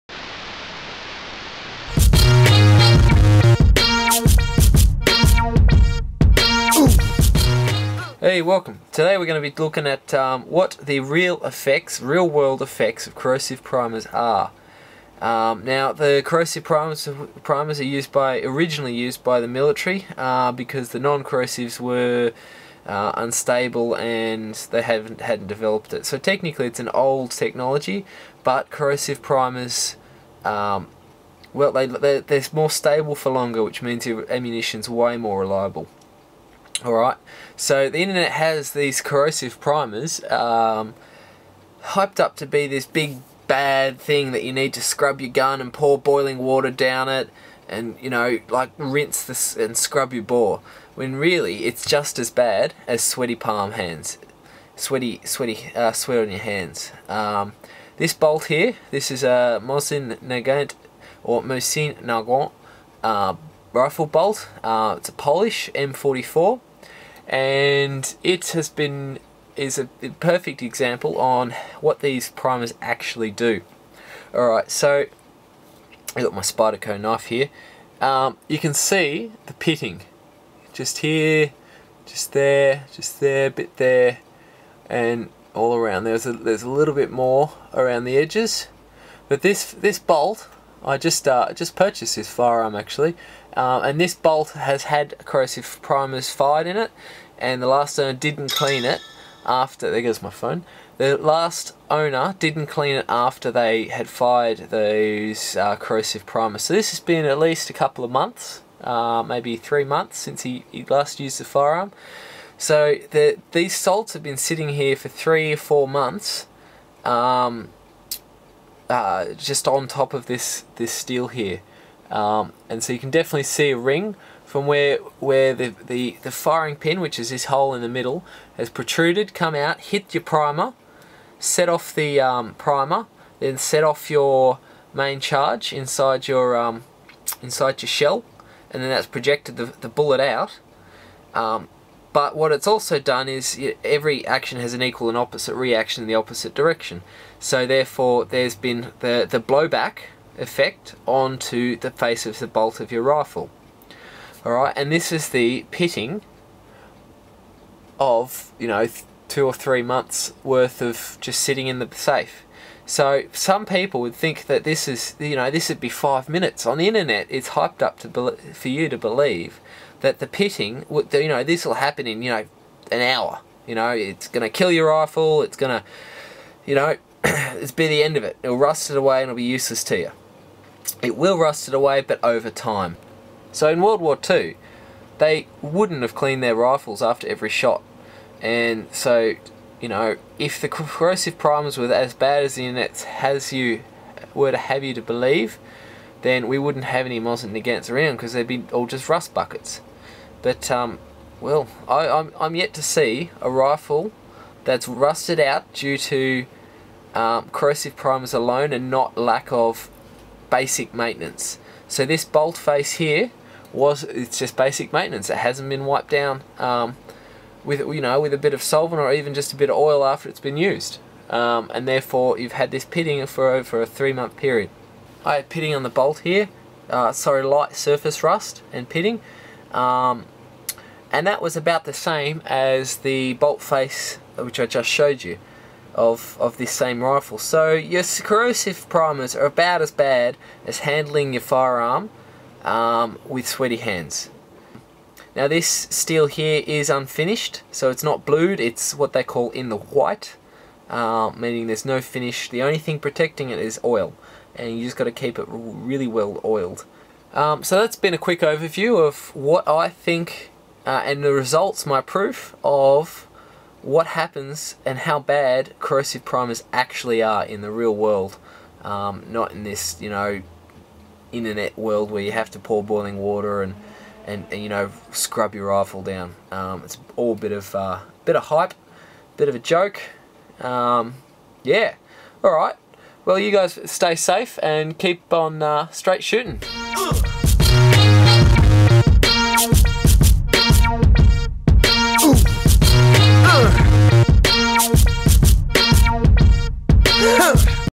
Corrosive Ammunition + Corrosive Primers And The Real World Effects
This Is a video of the real world effects of corrosive ammo.
It Isnt as bad as the internet has it hyped up to be.
You DONT need to pout water down the barrle of your rifle but simply push 1 wet patch through it followed by drying patches.
I am yet to see a firearm that has been 'rotted away' from corrosive ammo alone and not poor maintenance.

If you disagree please show me an example =]

As Usual, Constructive comments, Suggestions, Likes, Subscriptions are Welcomed and Appreciated!